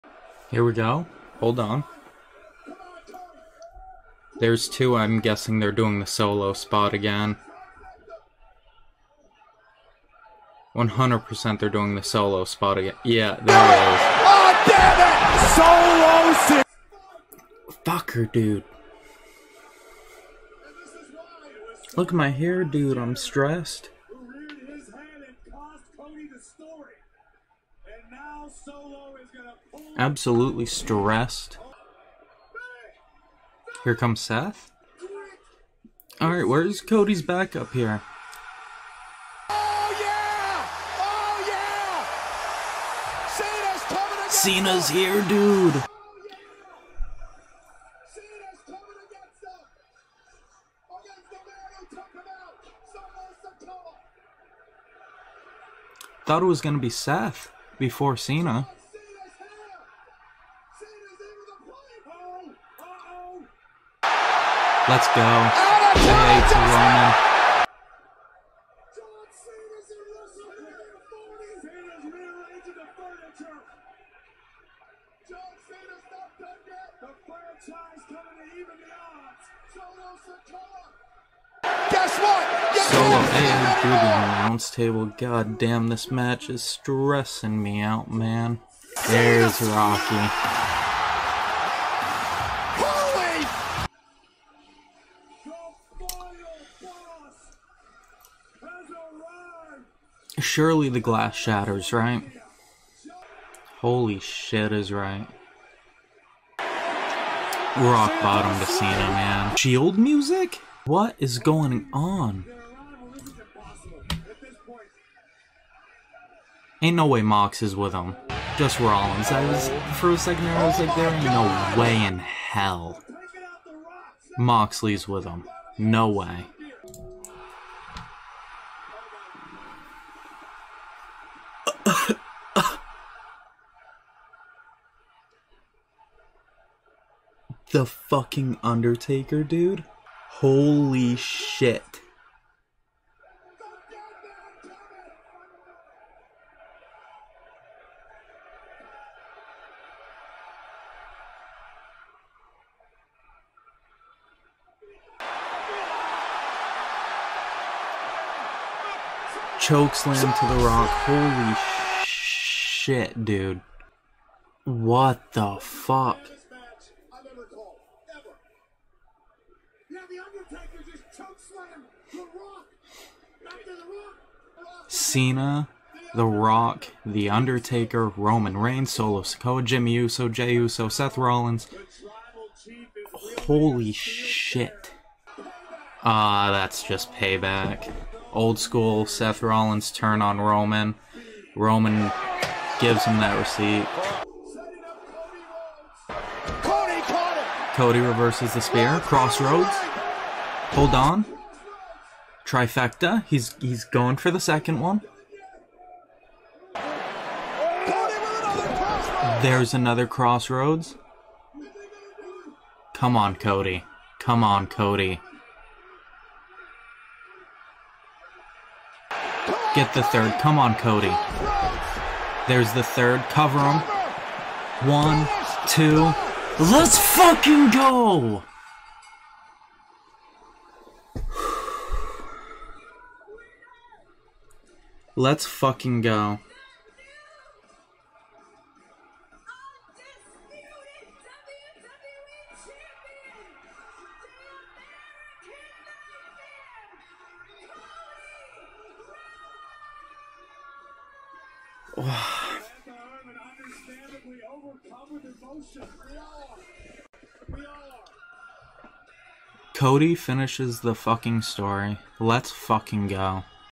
WWE WRESTLEMANIA XL: Cody Rhodes VS Roman Reigns | LIVE REACTION
Can Cody Rhodes overcome the odds & finish his story? Or will The Tribal Chief Roman Reigns finish it for him?